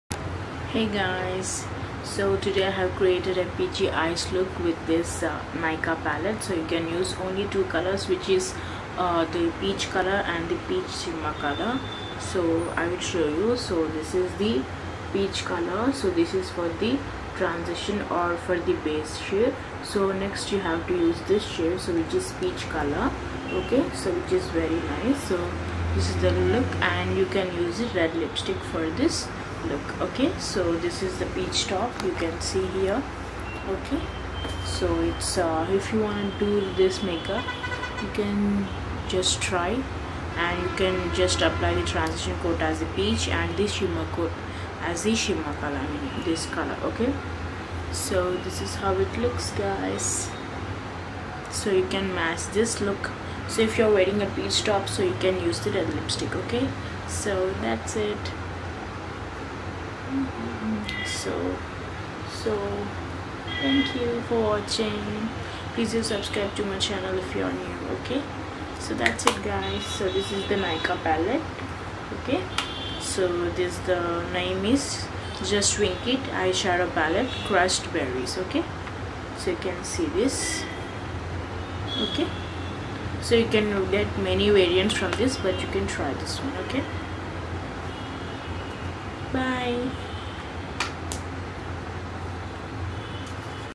Peach 🍑 Eye Makeup Using Nykaa Crushed Berries Eyeshadow palette || Sangeetha phebe
youtube channel for more such Amazing videos &🔔 Hit the Notifications bell ,so you don't miss any of My New videos